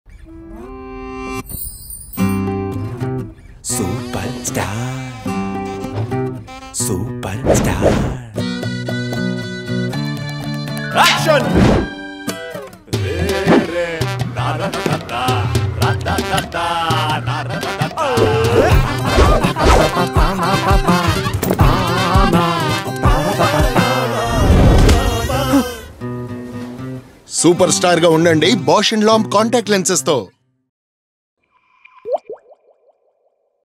Be A Superstar with B+L Contact Lenses​ - Telugu
​There was a Daydreamer, who dreamt of becoming like his favourite Superstar.​

But his path wasn't easy after all.​

So here's his story. Come, meet him and find out how to BeASuperstar!​